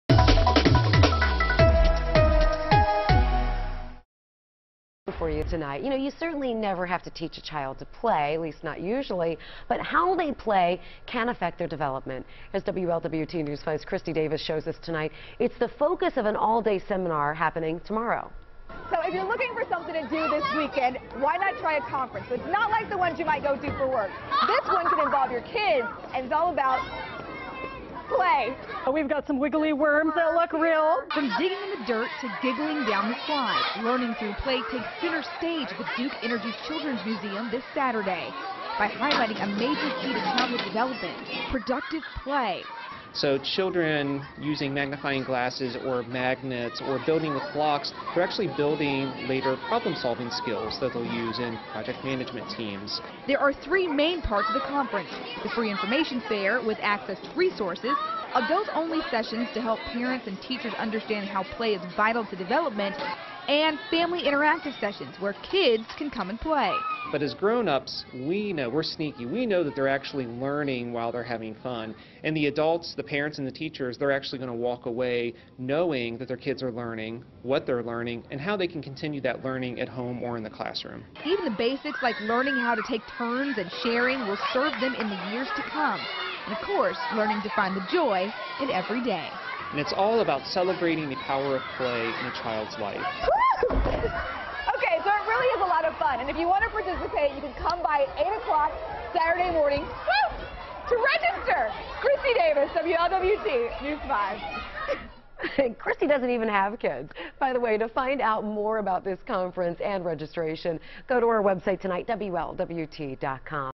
Program teaches kids how to learn through play
From digging in the dirt to cruising down a slide, the one-day conference will teach parents and teachers how to use productive play in childhood development.